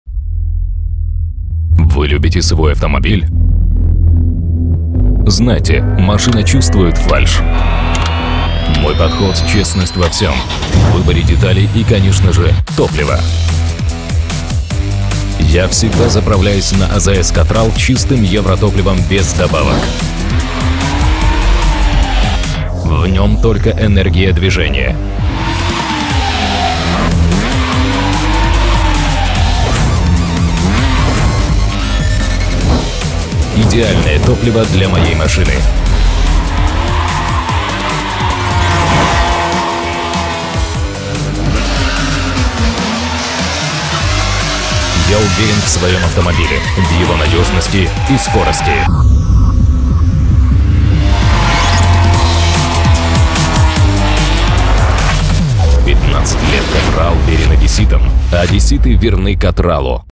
Katral -- We're Odessa's Citizens.
TV advert for Katral Petrol Company.
Director: Aleksandr Slyadnev
Camera:
Aleksandr Slyadnev
Konstantin Borisov (Helicopter camera)
Dima Zaburunnov
Iura Atom
Driver: Vladimir Palariev (Top Drift Pilot of Ukraine)
Post-Production: Denis Lazutkin
Sound Director: Murat Makoyev
Musical track author: Dmitriy Lazutkin (ColorBalance)
Thanks for the help:
Mikhail Palariev for the idea.
Max Taftay for the consultation.
InSound for the voice.
Mamai Production and TovarNaDom for the equipment.
Dmitriy Kot for the text
Aleksandra Kalach for the English translation.